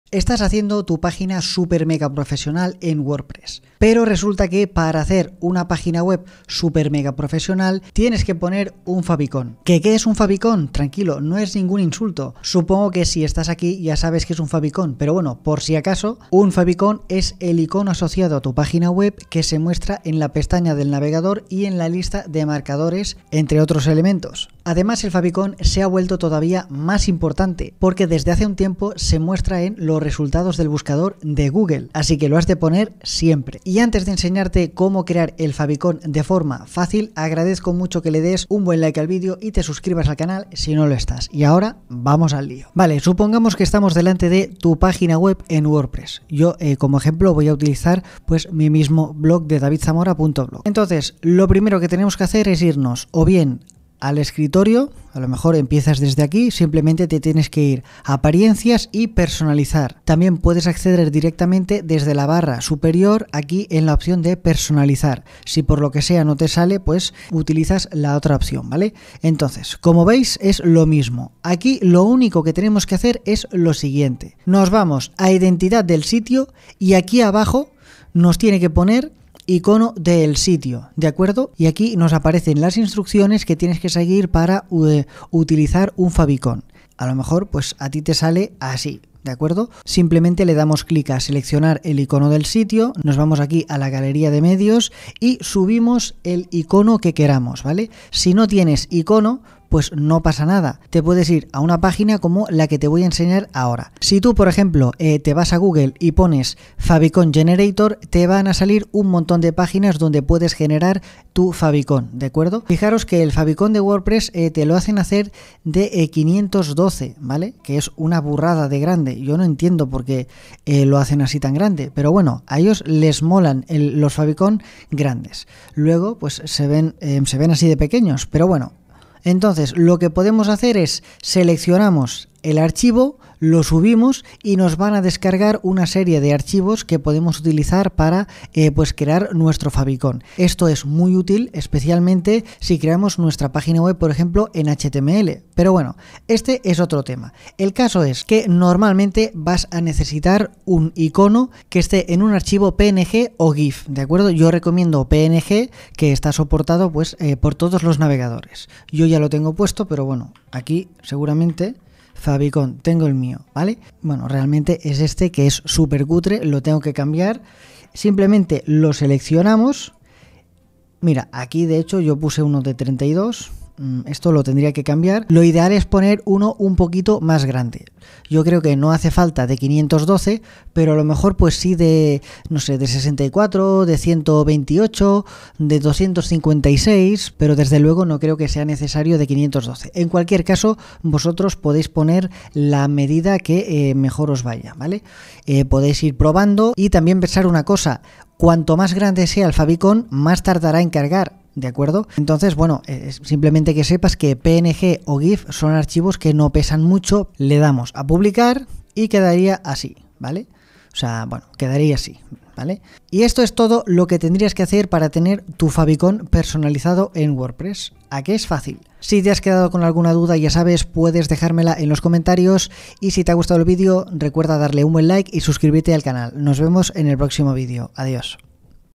Cómo cambiar el favicon en Wordpress (modo ultra-fast)
Si quieres tener una página web profesional en Wordpress debes utilizar un favicon. Así que hoy vamos a hablar de este pequeño pero crucial detalle: el favicon.

¿Sabes qué es un favicon?

Es ese icono asociado a tu página que se muestra en la pestaña del navegador y en los marcadores. Además, ha ganado más relevancia ya que aparece en los resultados de búsqueda de Google.

En este tutorial, te guiaré paso a paso para que puedas añadir o cambiar el favicon en tu sitio de Wordpress. Desde seleccionar el tipo de archivo adecuado (PNG, GIF o el clásico ICO), hasta ajustar las dimensiones correctas de tu imagen.
¿No tienes un icono o logo aún? No te preocupes, te mostraré cómo crear uno fácilmente utilizando herramientas en línea como Real Favicon Generator.